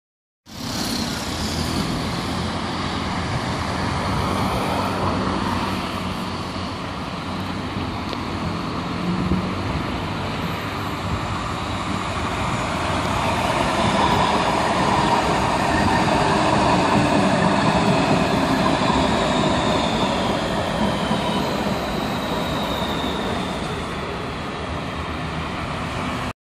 RET Metro 3*Combi Type B (5315) komt aan nabij Rhoon richting De Akkers
RET Metro Type B (MG2/1) rijtuigen: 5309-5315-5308 komt aan nabij Rhoon richting De Akkers op lijn D.

Metrorijtuig 5315 na de Mid-Life Revisie.

Materieel: Driewagentrein MG2/1
Station: Rhoon
Lijn: D richting De Akkers
WD: 921
Tijd: 16:24
Data: Woensdag 22 oktober 2014